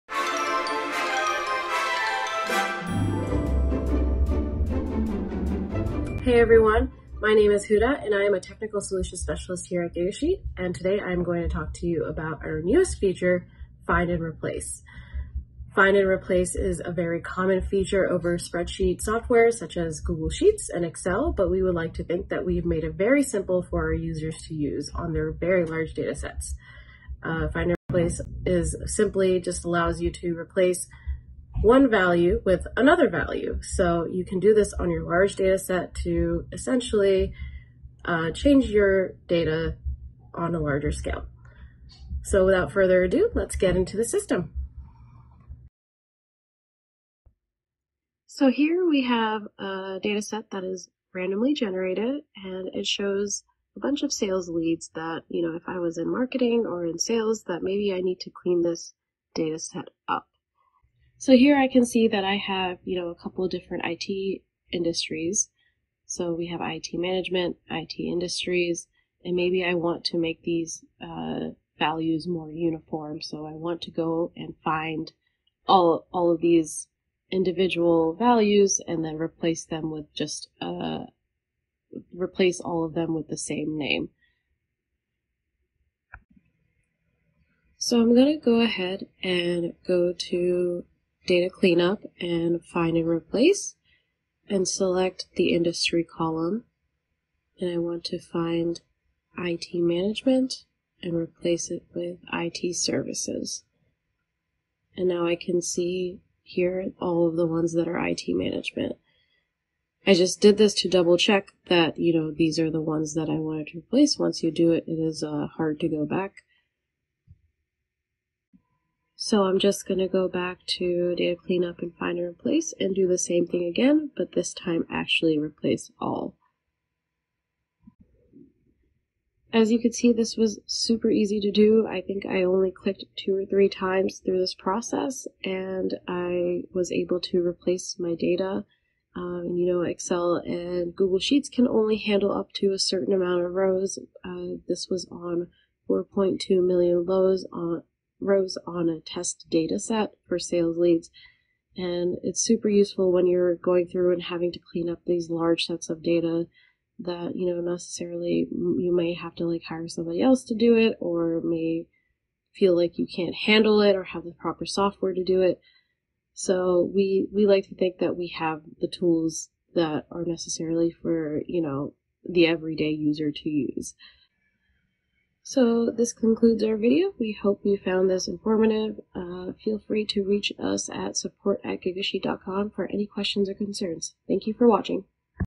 Find & Replace In Huge Spreadsheets, Too Big For Excel
Need to know how to find and replace values in a huge spreadsheet, CSV, or JSON file? Look no further than Gigasheet! Anyone can easily find and replace values in massive files using these simple steps. Create your account free at gigasheet.com